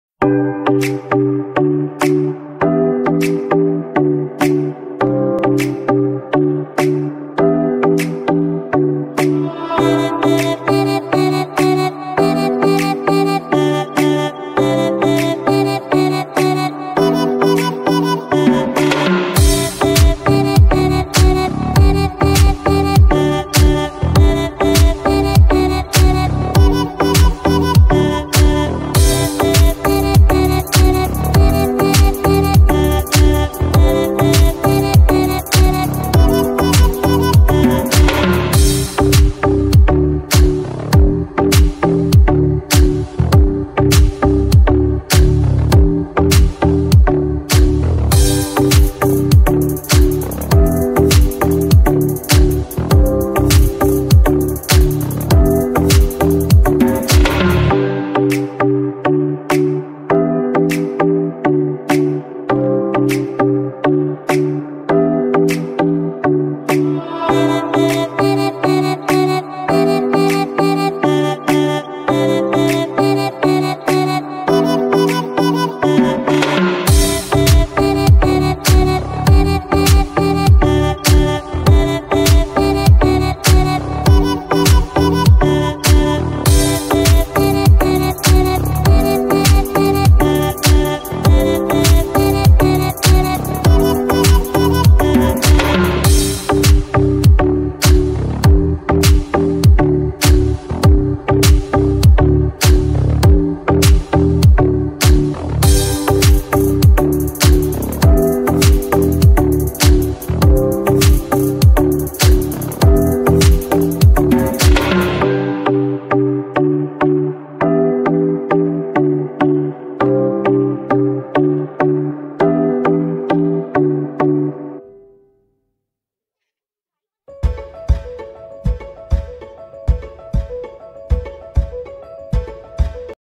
Diya making with paper/ 3D paper diya🪔 decoration/ Diwali decoration ideas
Hello everyone 😀
Welcome back to my new video this video is  on Diwali decoration with paper...
Colourful papers (any colour)
Glue
Gillter paper (optional)

Bye bye 💖